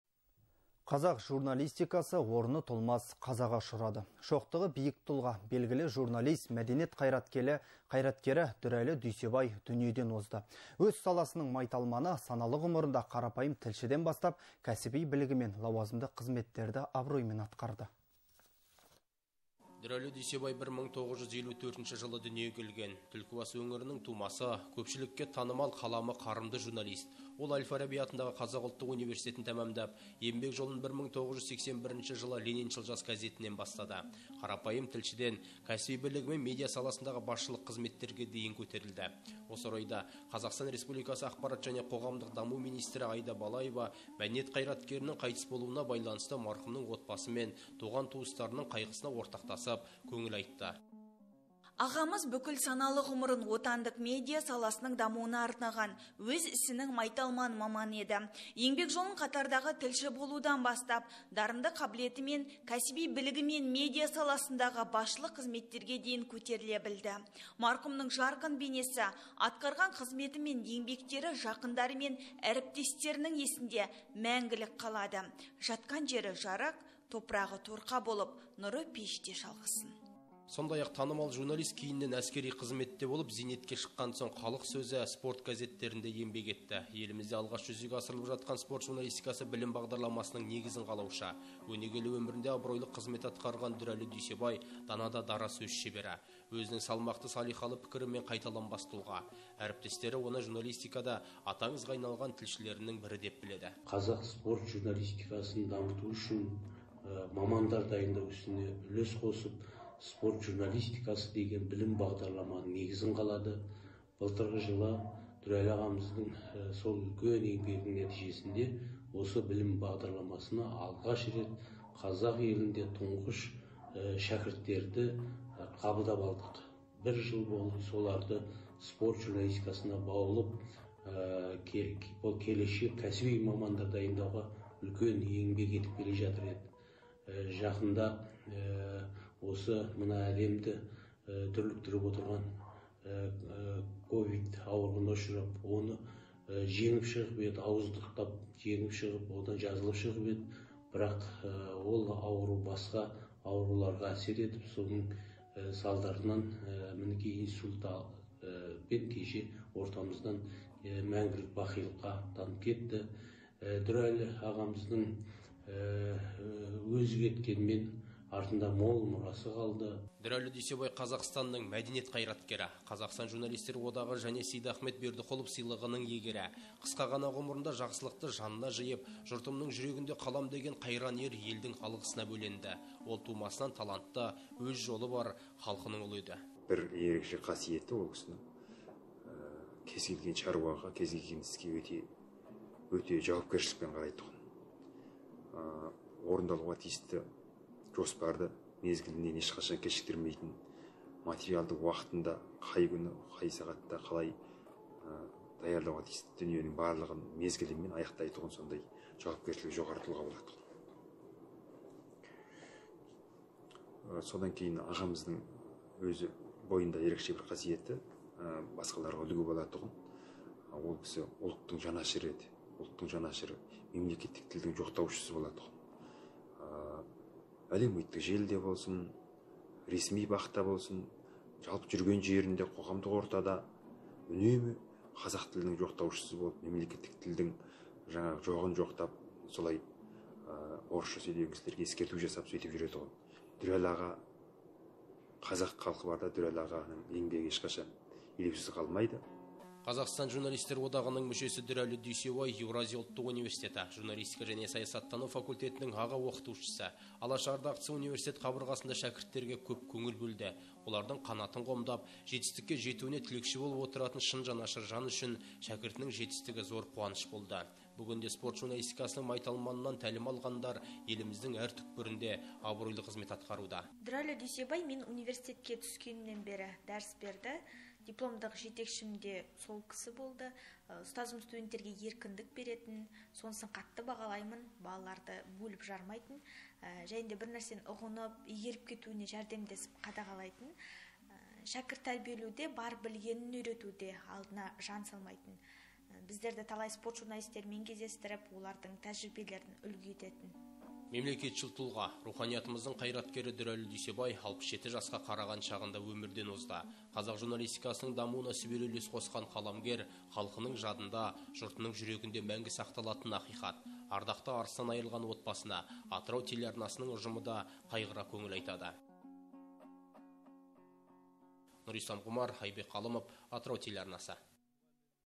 ЖУРНАЛИСТ, ҰСТАЗ ДҮРӘЛІ ДҮЙСЕБАЙ ДҮНИЕДЕН ОЗДЫ
Қазақ журналистикасы орны толмас қазаға ұшырады. Шоқтығы биік тұлға, белгілі журналист, мәдениет қайраткері Дүрәлі Дүйсебай дүниеден озды. Өз саласының  майталманы саналы ғұмырында қарапайым тілшіден бастап, кәсіби білігімен лауазымды қызметтерді абыроймен атқарды.